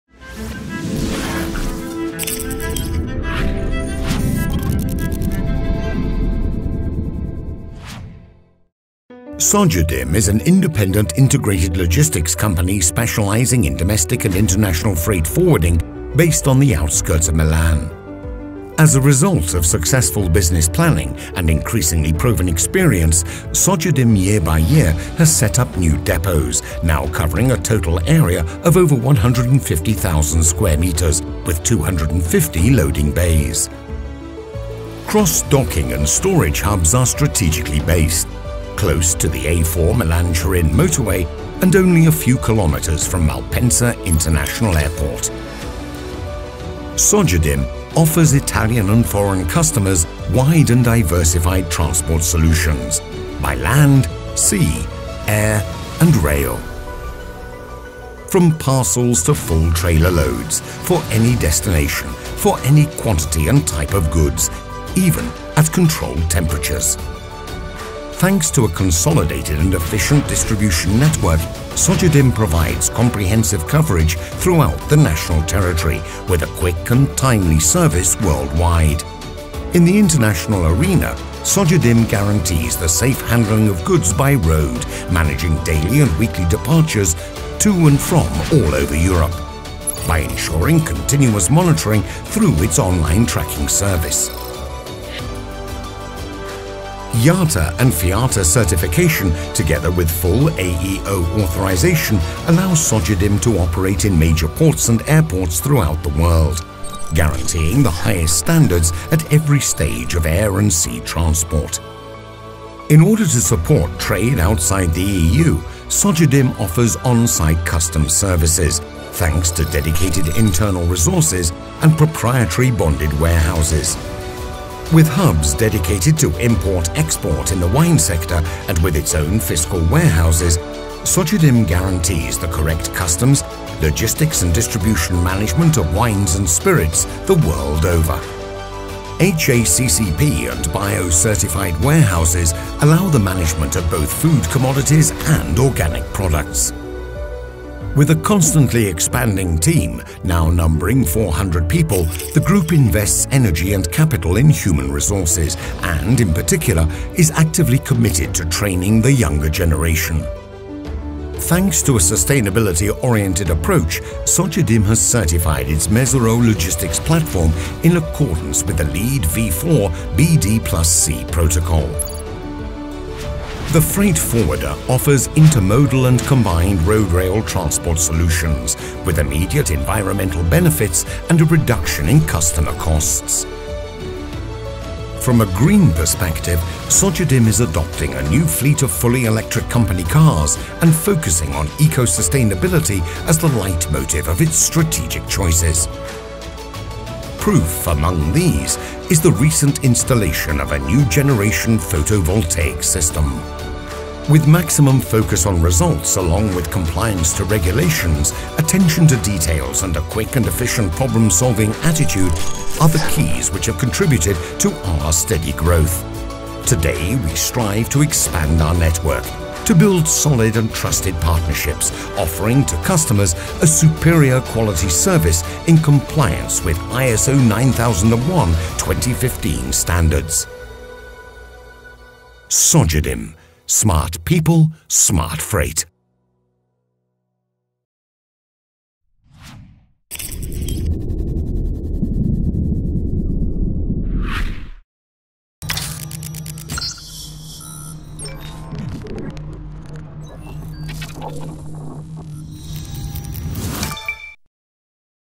Sogedim | corporate video | english
The new corporate video describes the way how the worldwide freight forwarder Sogedim plans, operates and monitor international transports solutions.
Sogedim offers Italian and foreign industry wide and diversified transport solutions, by land, sea, air and rail.
The Road freight forwarding range is stronger thanks groupage solutions.
IATA and Fiata certification together with full A.E.O authorization, allow Sogedim to operate in major ports and airports throughout the world, guaranteeing the highest standards at every stage of air and sea transport.
500,000 shipments per year - 10,000 clients - 400 employees - 150,000 sqm warehouses.
Referral partner for integrated logistics and specialized transport solutions.
The best staff at your service, for fast and on-time deliveries in Italy and abroad.